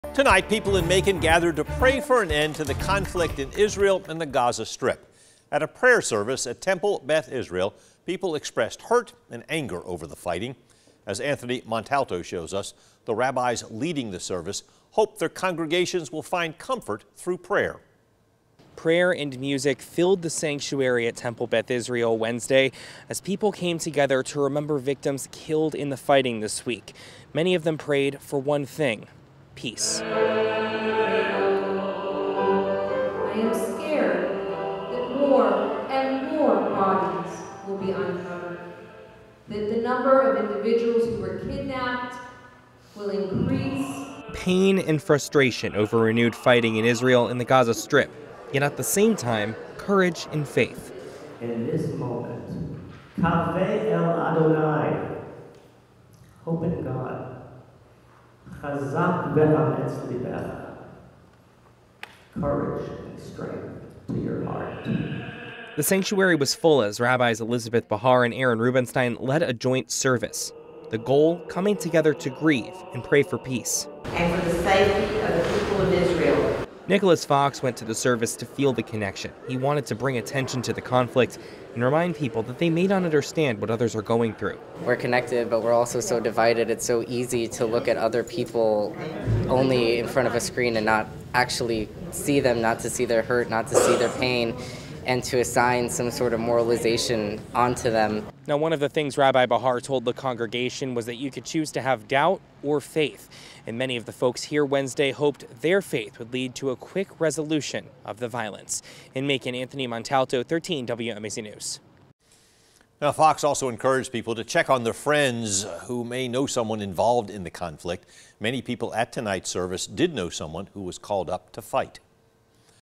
Georgia rabbis pray for an end to violence in Israel and Gaza
Temple Beth Israel hosted a joint prayer service to give people space to grieve and process.